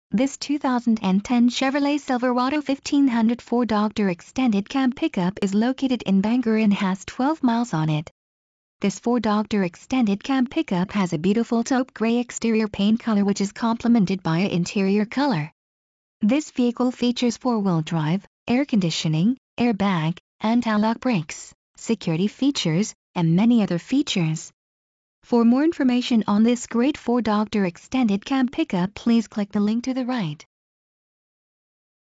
2010 Chevrolet Silverado 1500 La Crosse WI WI
Price: $ 32,999
Mileage: 12
Body Style: 4 Dr. Extended Cab Pickup
Ext. Color: Taupe Gray
Int. Color:
Engine: 5300 V8
Drive Type:
Fuel Type: Flexible Fuel
Doors: 4
Stock No.: 159483
VIN: 1GCSKSE37AZ159483